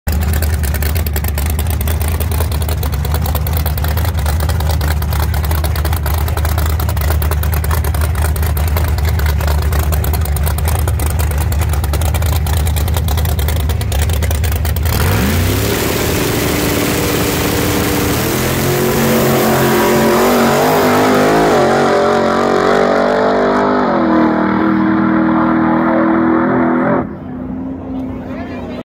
nuevo Récord en chihuahua🇲🇽 8.05 en el cuarto de milla 🚦 Mustang Shelby gt350 🔥
excelente pasada de este mustang shelby en chihuahua capital